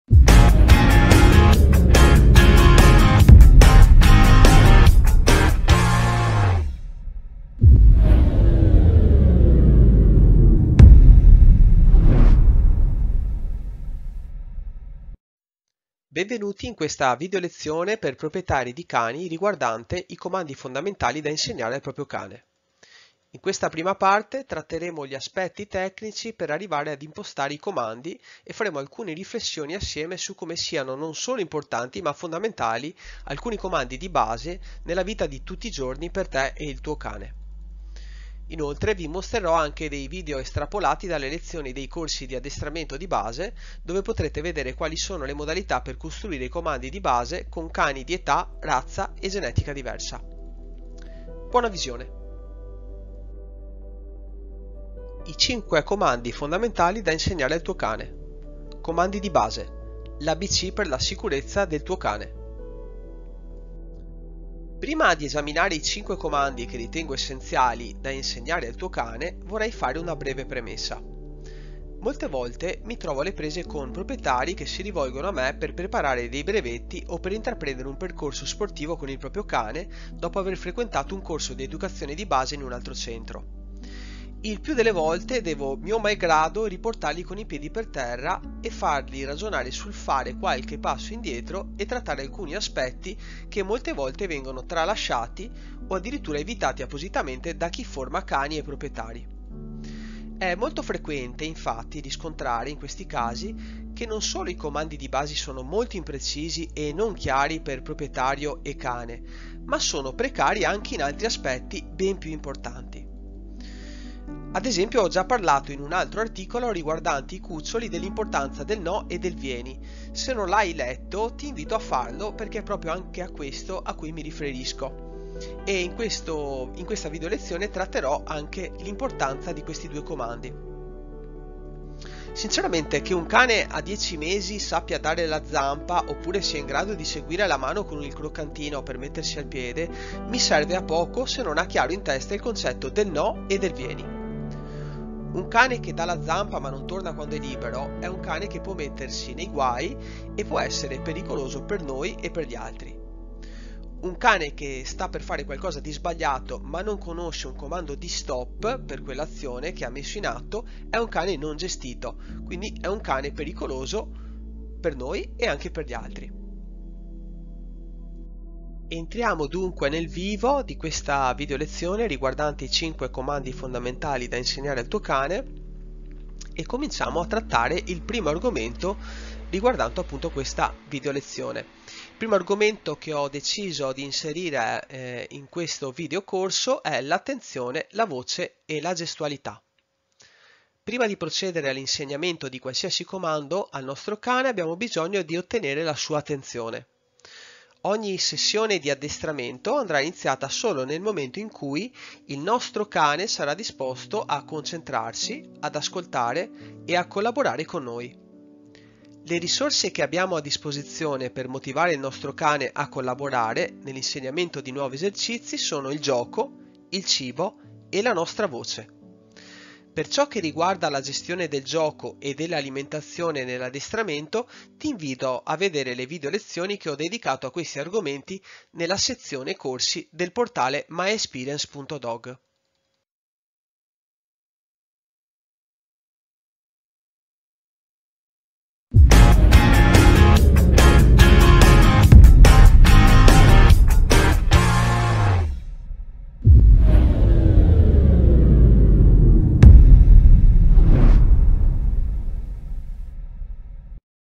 VIDEOLEZIONE I 5 COMANDI FONDAMENTALI DA INSEGNARE AL TUO CANE - myexperience.dog
comandi di base l' "ABC" per la sicurezza del tuo cane

Argomenti della Videolezione:
Introduzione;
L'Attenzione, la Voce, la Gestualità;
Il "NO";
Il "VIENI;
Il "SEDUTO";
Il "TERRA";
Il "RESTA"